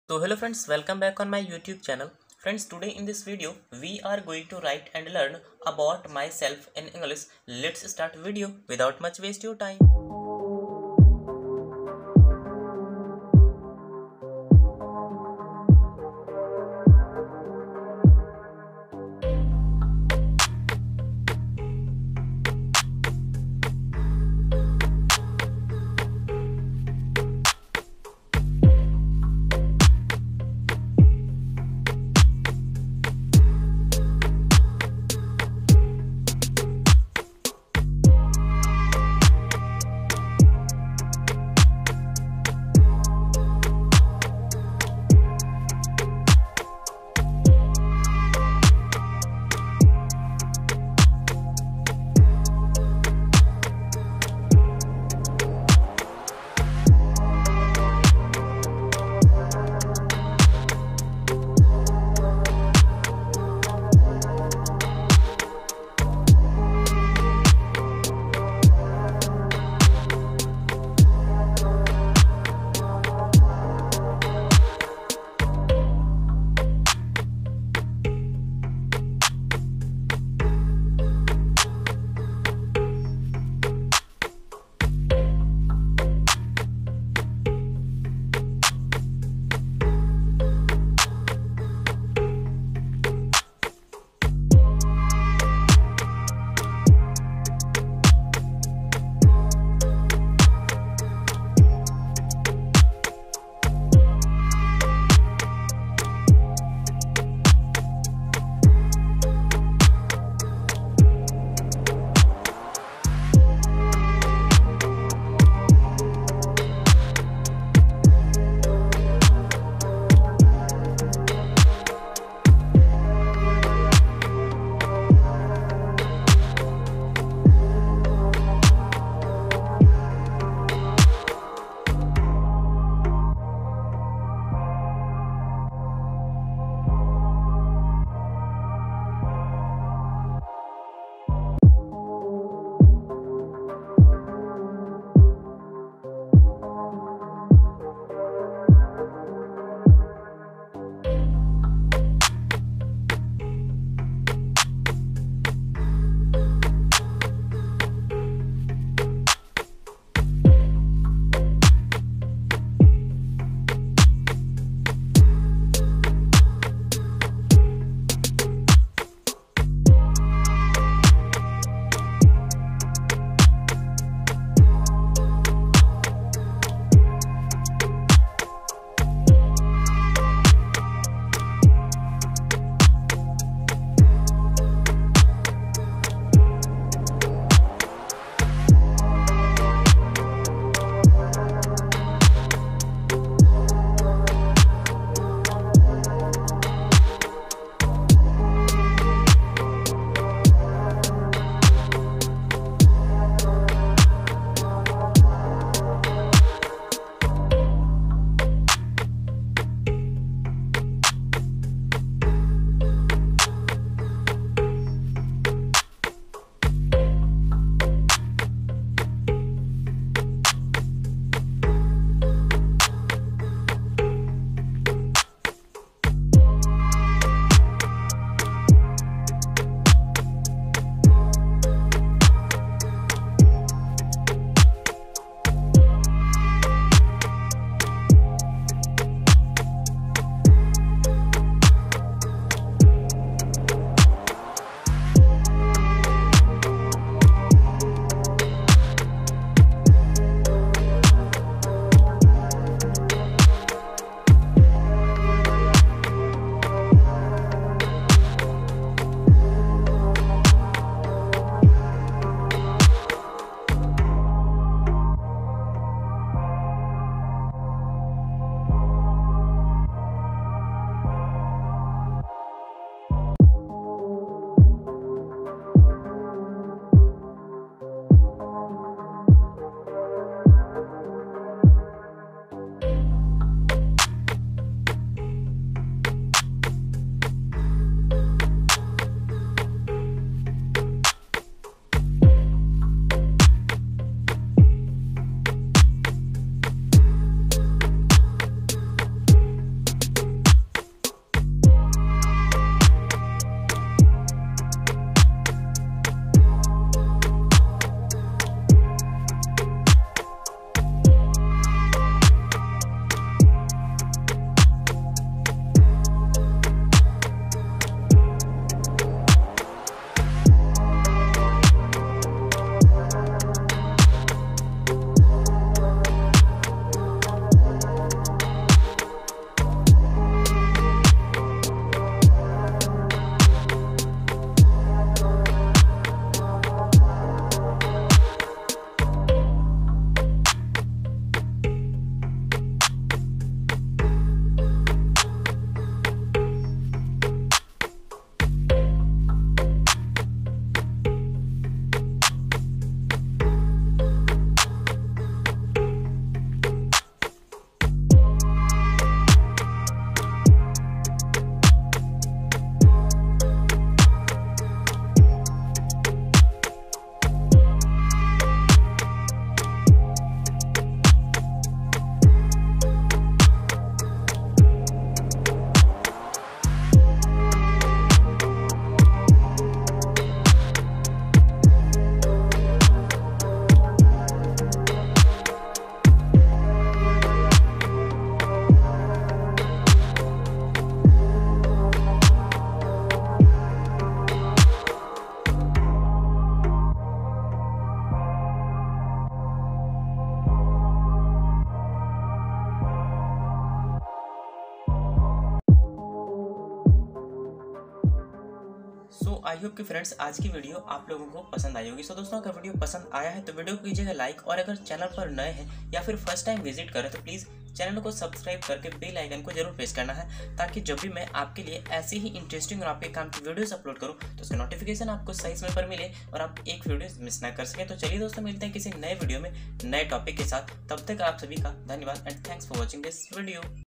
Myself essay in english / About Myself / Essay on Myself in English writing learn
Friends, Today in this video you will learn how to write essay on myself in english with my beautiful and print handwriting . Everytime teacher asked their students to write about yourself. This video is all about to write myself paragraph ✍️.

✍🏿I hope that you will learn and enjoy watching this helpful video🌷.

lot of ❣️❣️❣️